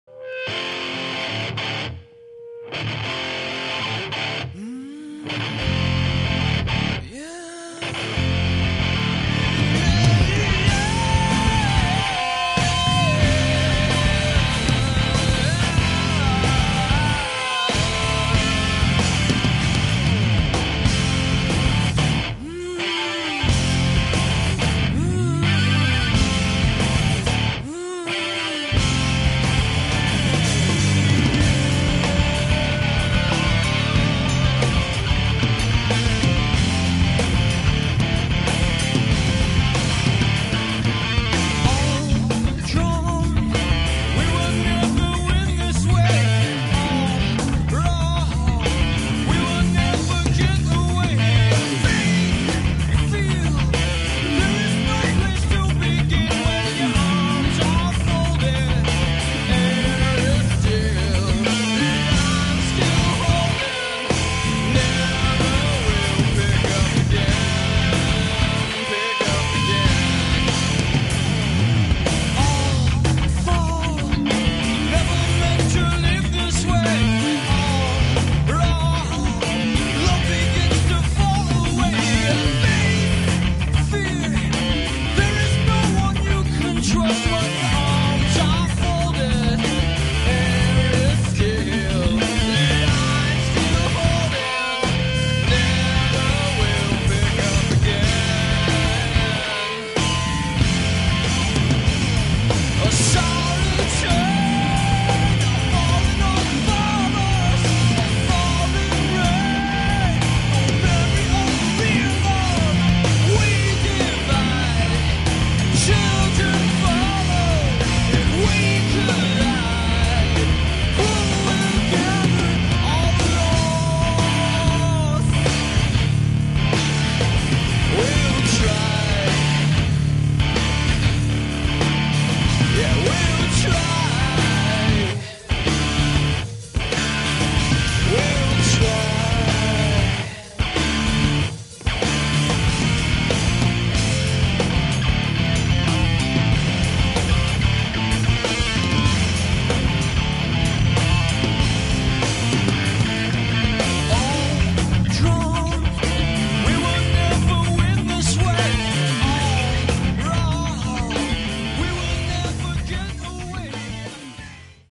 Space ace- DSIWare on 3DS
I never played this so i'm not good. By the way the music is from a band that was a friend of my fathers about 7 years ago who broke up about the same time. I just found the disc laying around my house and figured i'd throw the track on there. They were called "Sun aged to rain"(Not sure how that's possible but that was the name).Also i didn't keep the real audio because it was too noisy. I tried to save it but no beans. =(